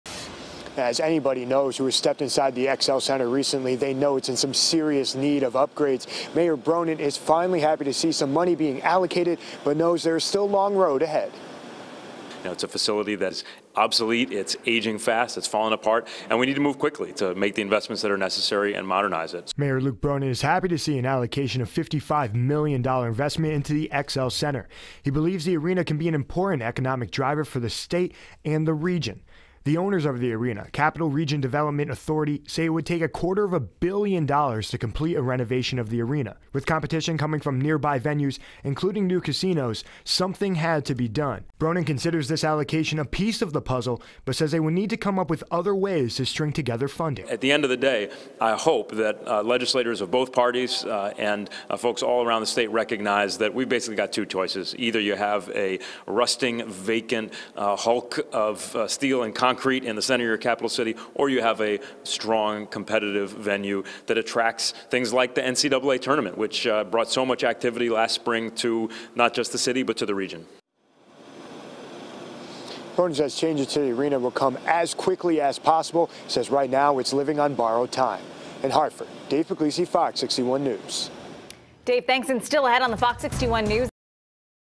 Gov. Lamont proposes to invest $55 million into XL Center
HARTFORD - Governor Ned Lamont filed for a bond package that will include $55 million-dollars for renovations to the XL Center.